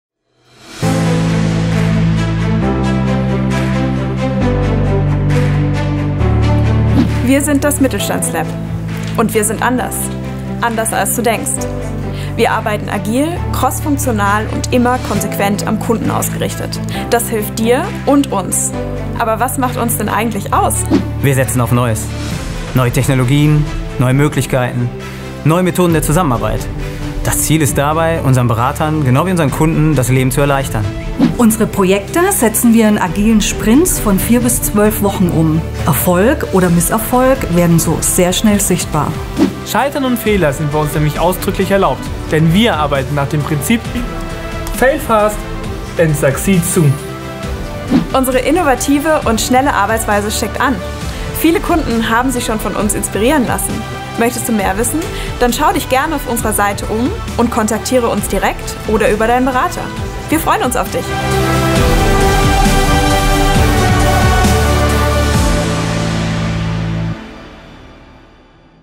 Mittelstands-Lab - Das Innovationsteam im Firmenkundenbereich der Commerzbank
Willkommen im Mittelstands-Lab - Wir sind das Innovationsteam im Firmenkundenbereich der Commerzbank.

Wir wollen Euch inspirieren und Euren Geschäftsalltag leichter machen mit innovativen und bedarfsgerechten Produkten und Prozessen.

Lerne uns kennen!